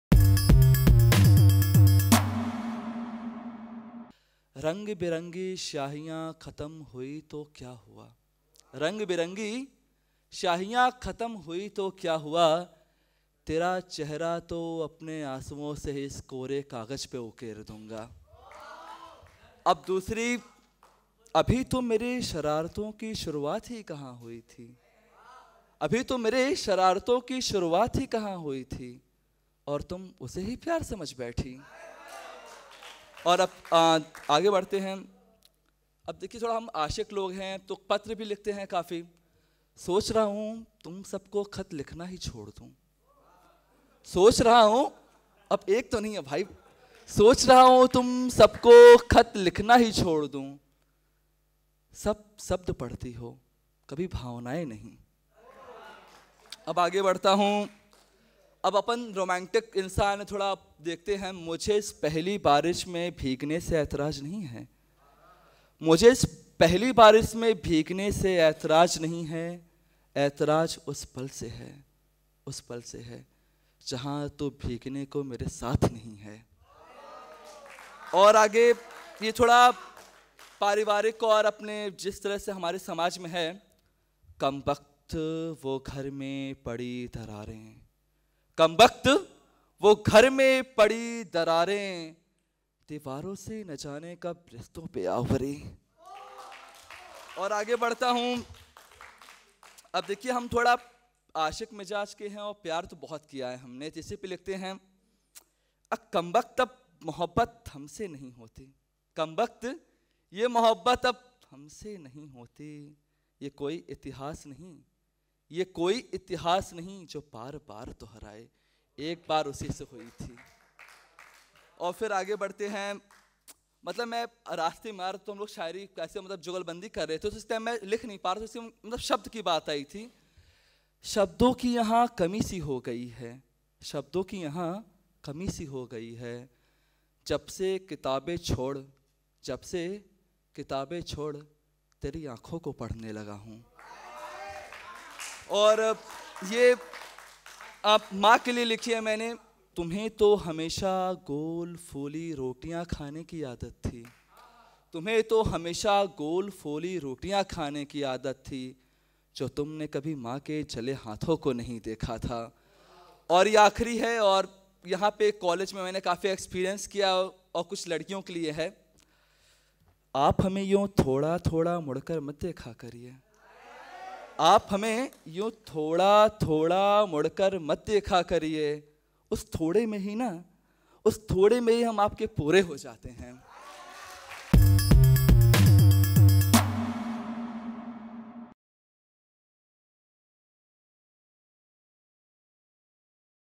Romantic Shayari by IIT BHU Student | Alcheringa 2018
Watch Vikas Pandey, a student of IIT BHU Varanasi, perform his shayari at IIT Guwahati's cultural festival 'Alcheringa 2018'.